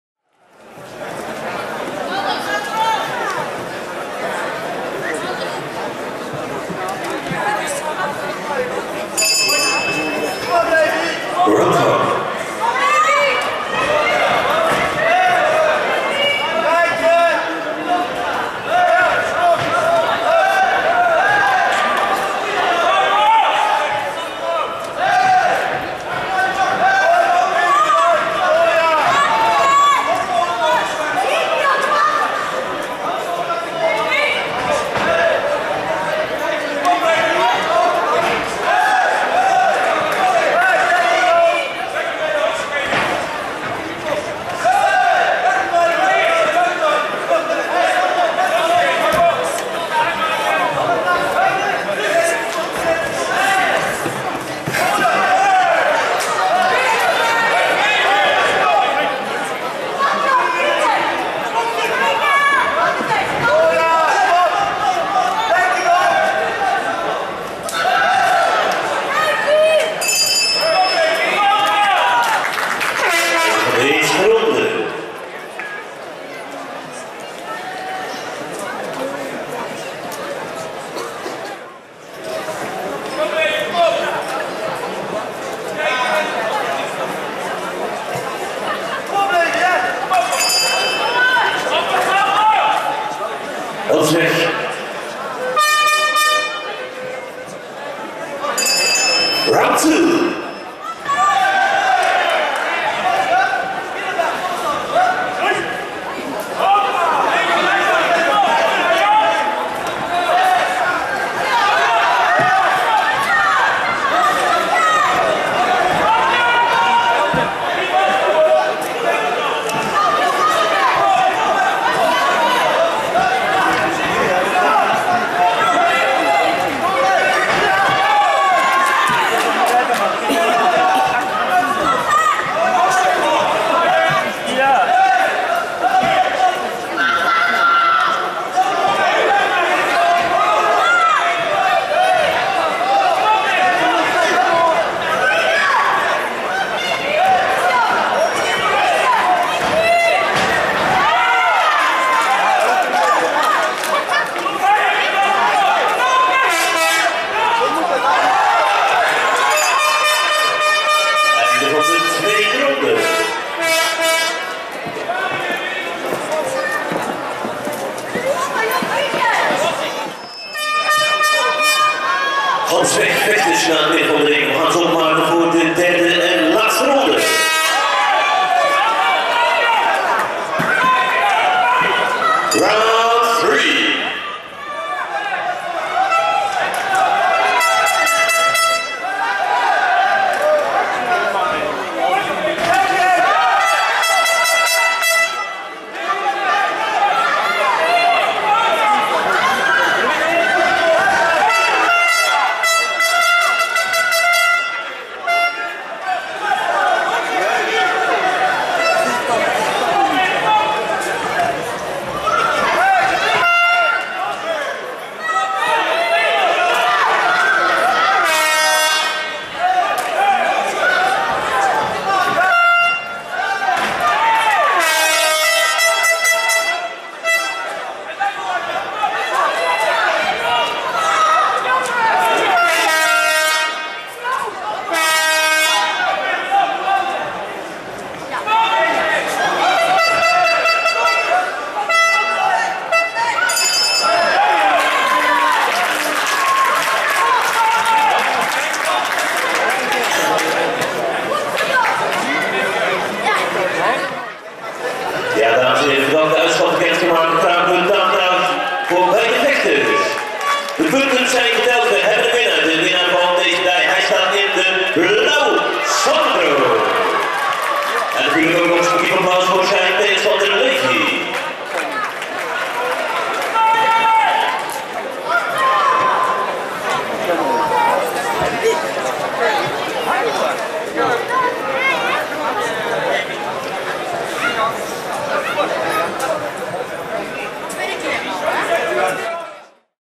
Levi Mikkers vs Sandro Eijkenboom
Junior fight kickboxing 3 x 1 min Levi Mikkers (Golden Glory Helmond) vs Sandro Eijkenboom (Yego Gym) @ Kickboxing event Natural Powers - Golden Glory Helmond, Sporthal Haagdijk Eindhoven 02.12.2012.